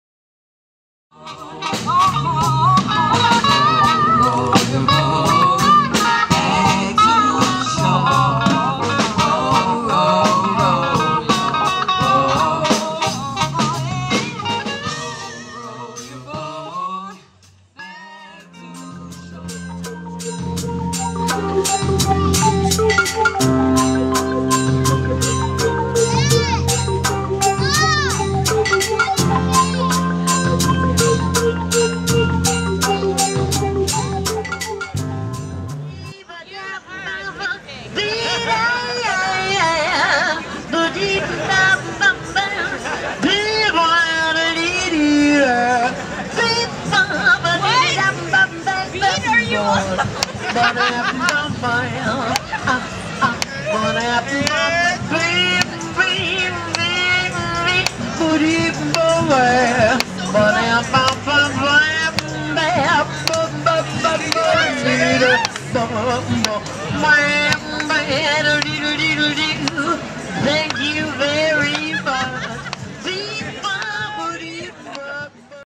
Dance like no one......
A couple of observations during my visit to Toronto